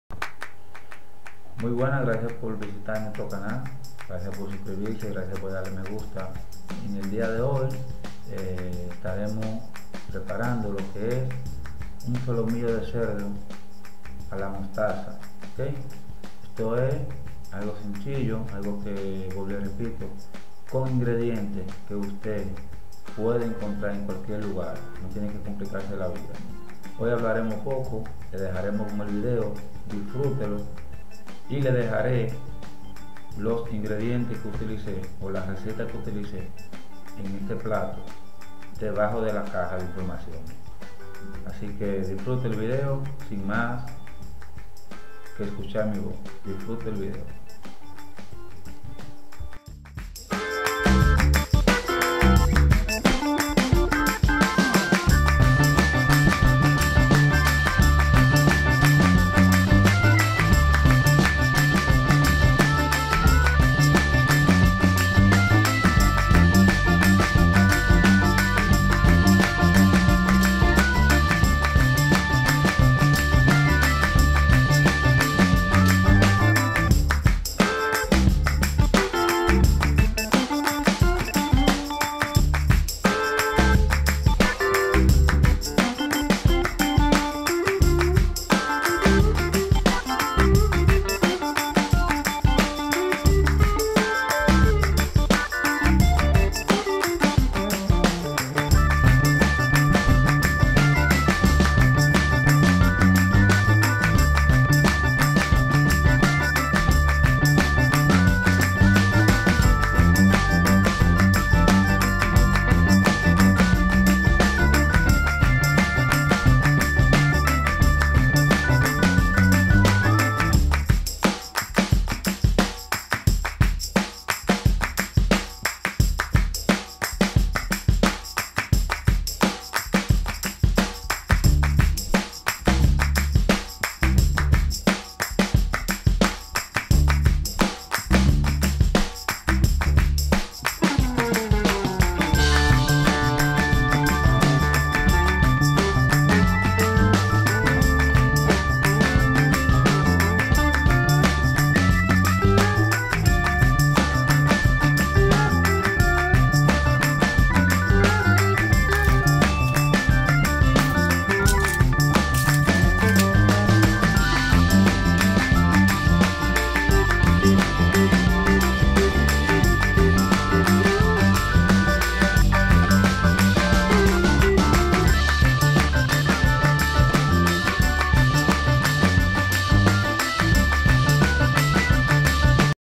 Los Dominicanos: Solomillo de cerdo a la mostaza
Recetas que vamos a comer hoy.
Solomillo de cerdo a la mostaza
Cerdo
Mostaza
Sal
Perejil
Ajo
Orégano
Pimientas
Queso rallado
Miga de pan
Pan rallado.

Facebook page. @puertoplatanews
Twitter. @puertoplatared
Instagram. @depuertoplata